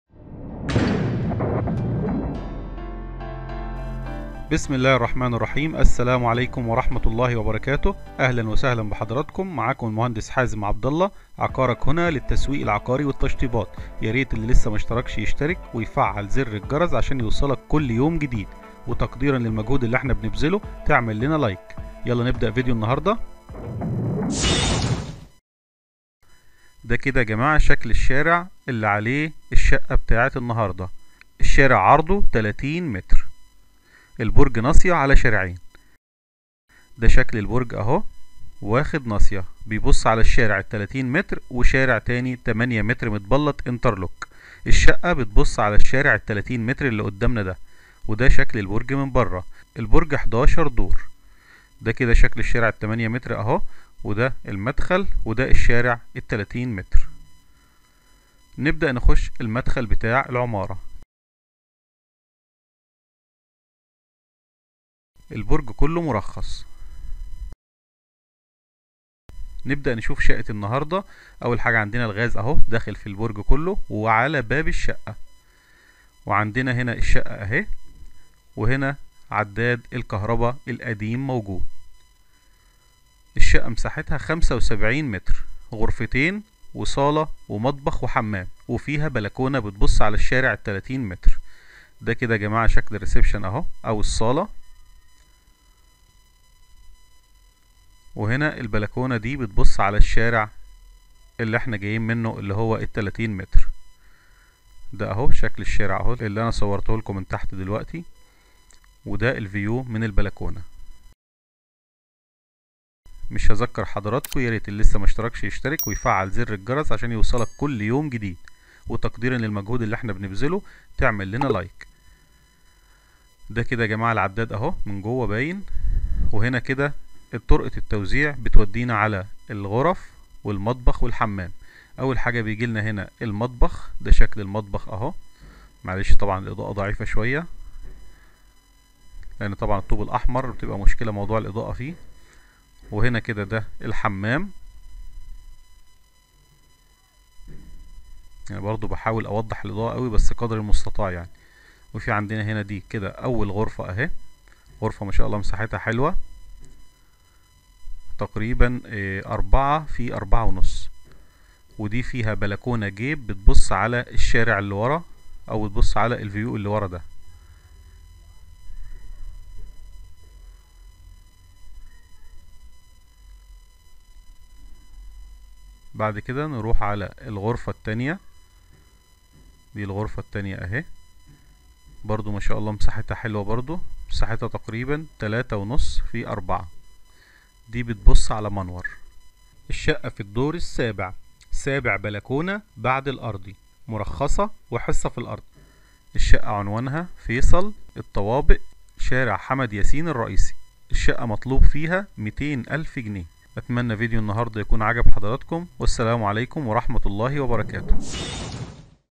شقة للبيع مرخصة و حصة فى الارض طوب احمر فقط 200 ألف جنيه - كود ( 1075 )
نوع العقار : شقة سكنى
المساحة : 75 متر ( تحت العجز والزيادة )
التشطيب : طوب احمر
عدد الغرف : 2
عدد الحمامات : 1
الطابق : السابع ( سلم )
الأسانسير :  لا يوجد حاليا و جارى جمع مبلغ 8000 جنيه من كل شقة لعمل أسانسير
عداد الكهرباء : قديم ديجيتال
عداد الغاز : على باب الشقة
عداد المياه :  مشترك مع العمارة
الترخيص : مرخصة + حصة فى الارض
العنوان : فيصل - الطوابق - شارع حمد ياسين الرئيسي
السعر : 200 ألف جنيه